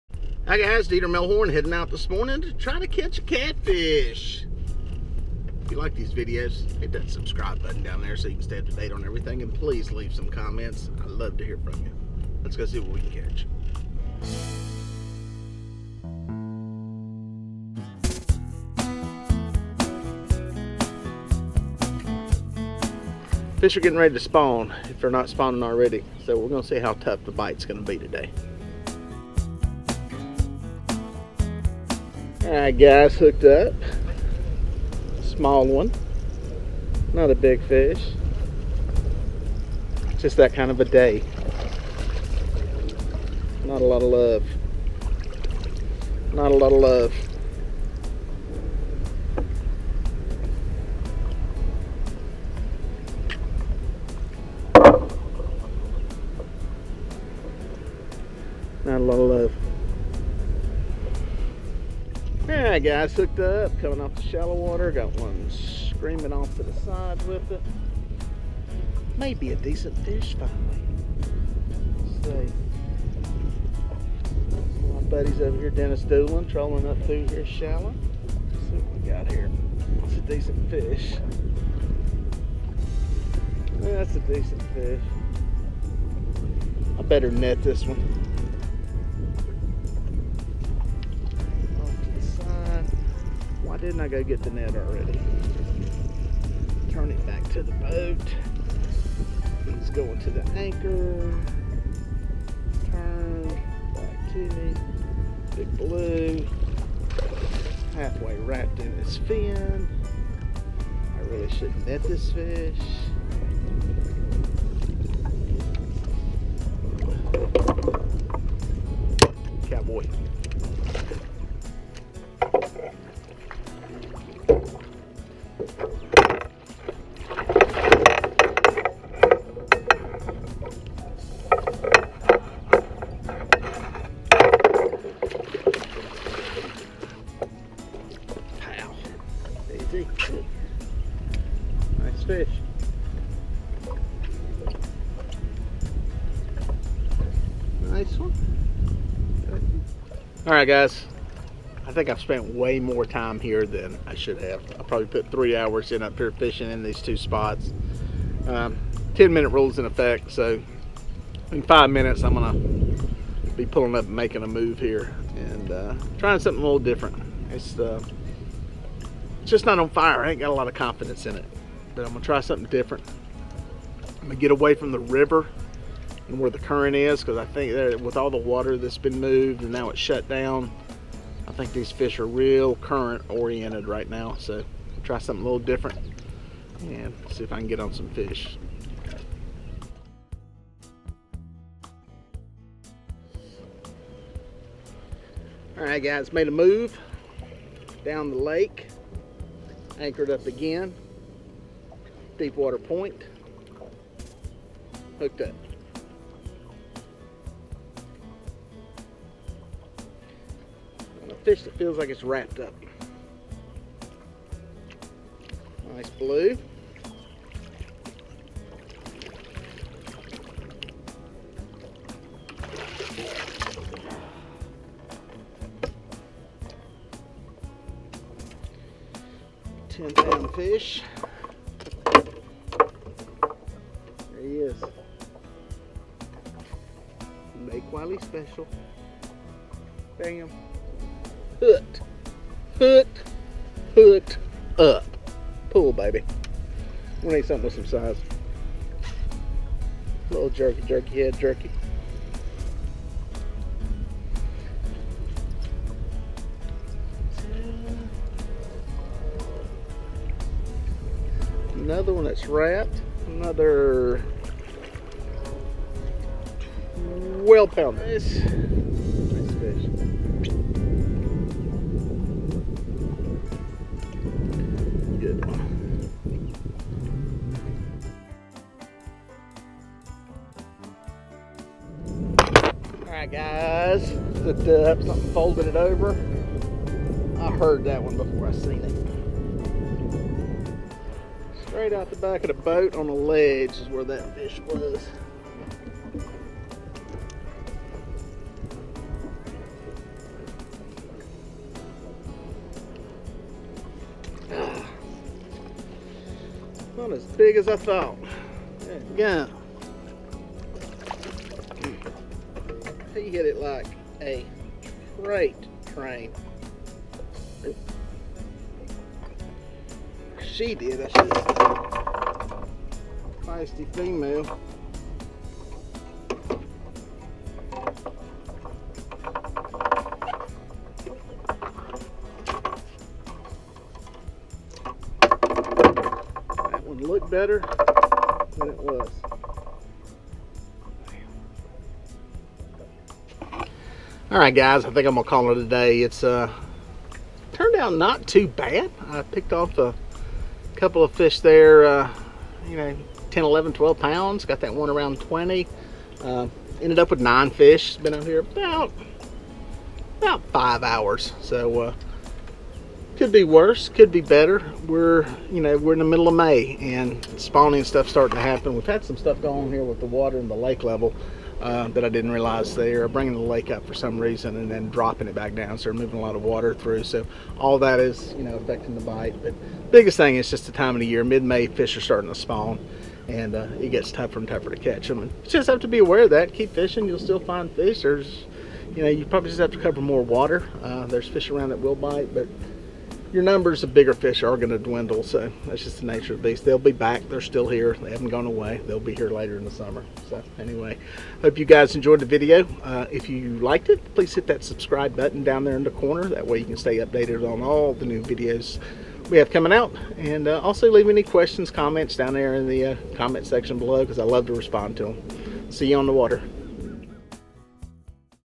How to Catch Catfish During the Spawn - Poor Catfishing Conditions
This catfishing trip has some good tips for catching catfish in the days just before the spawn starts to kick in.  Catching catfish can be tough during this time period, so keep this in mind when you are not catching a lot of catfish.

I try to cover a lot of water during the spawn.  Not all catfish spawn at the same time, so you can find areas where catfish are not locked up with mates.

Catfish will hit most any bait during this time period including shad, perch, bluegill and many prepared baits.

Drifting, trolling or bottom bouncing are effective methods for covering water and catching catfish during this time period.